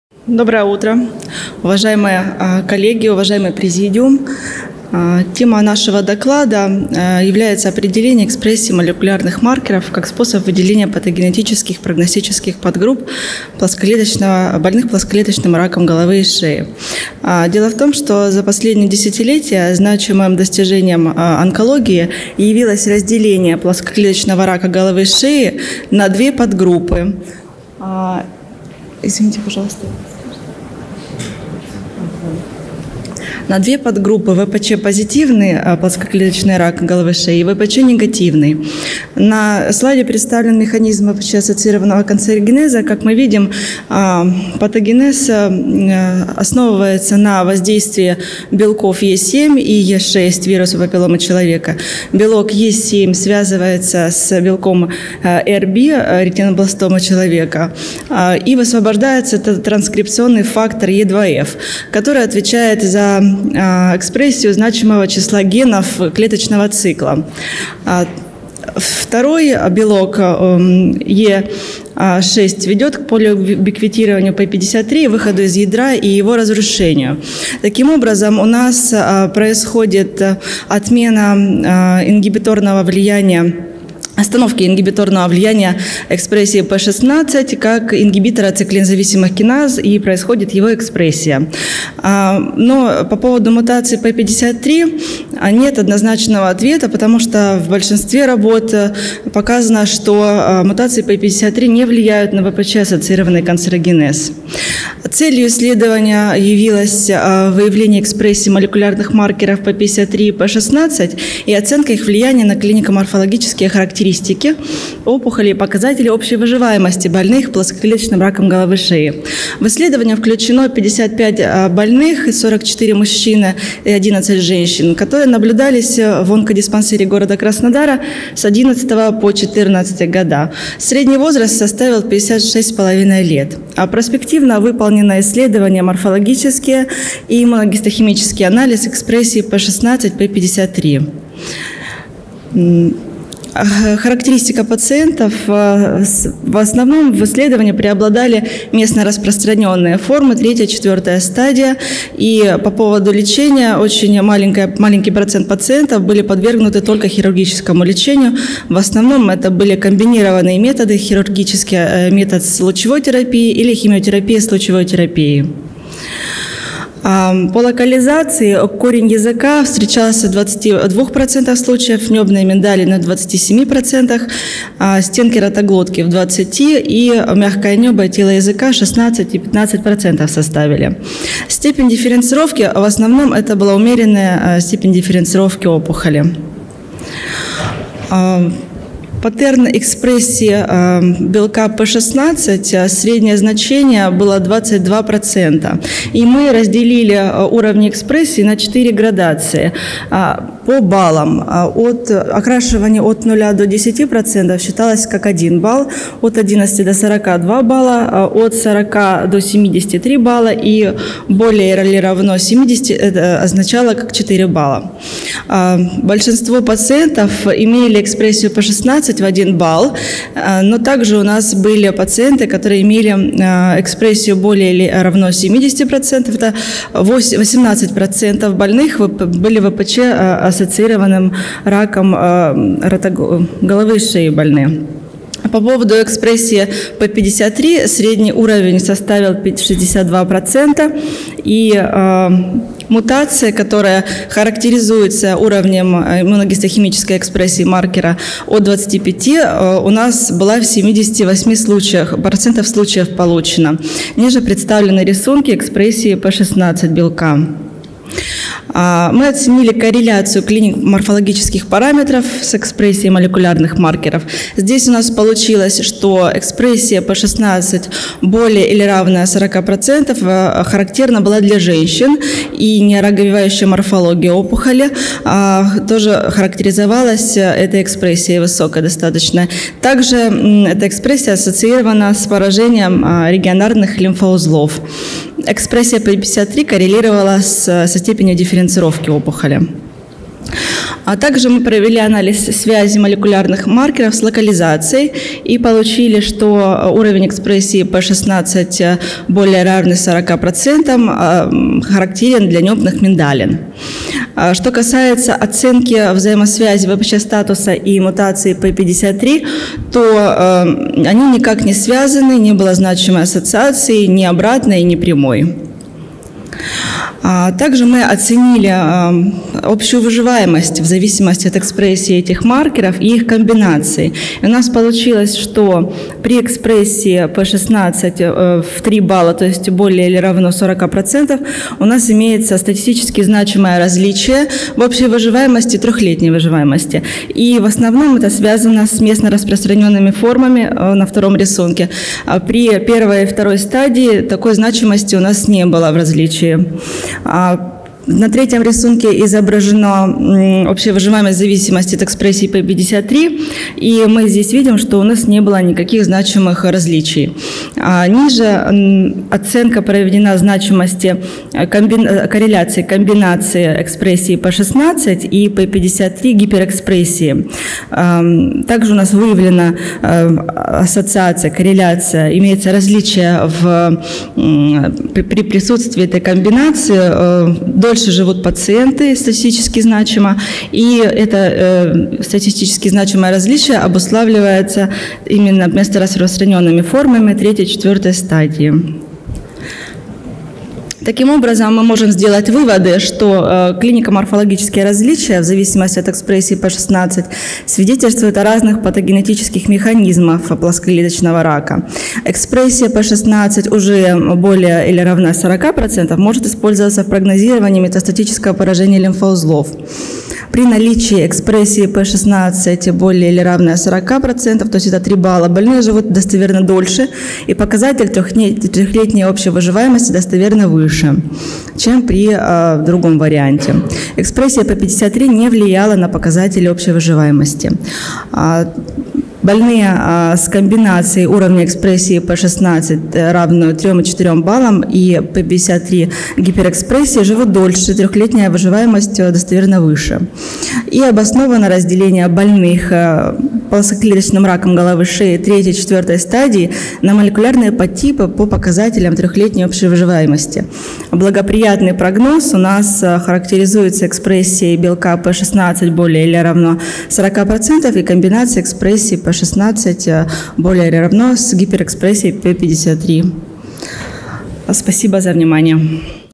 Определение экспрессии молекулярных маркеров как способ выделения прогностических подгрупп больных
Определение экспрессии молекулярных маркеров как способ выделения патогенетических и прогностических подгрупп больных плоскоклеточным раком головы и шеи
д-р Стукань Анастасия Игоревна, ГБУЗ «Клинический онкологический диспансер №1» Минздрава Краснодарского края, Краснодар
(XXII Российский онкологический конгресс, 13-15 ноября 2018, Москва)